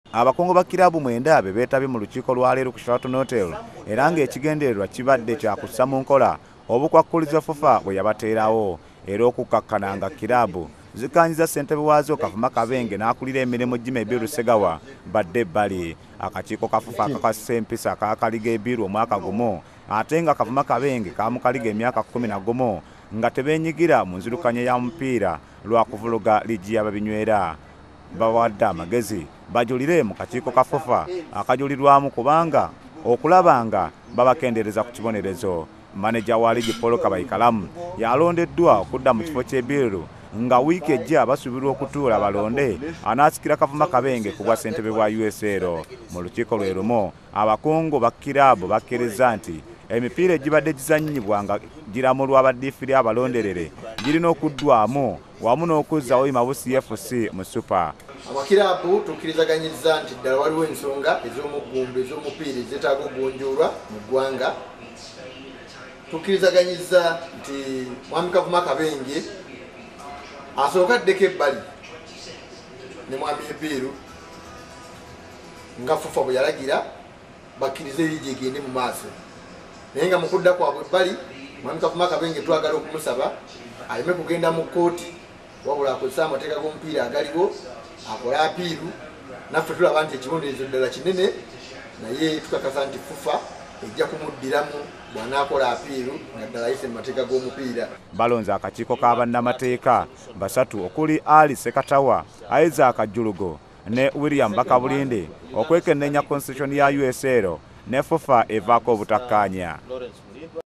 Kiraabu zigobye Kavuma Kabenge
Kiraabu za USL zigobye mukulu wa liigi Kavuma Kabenge ku biragiro bya FUFA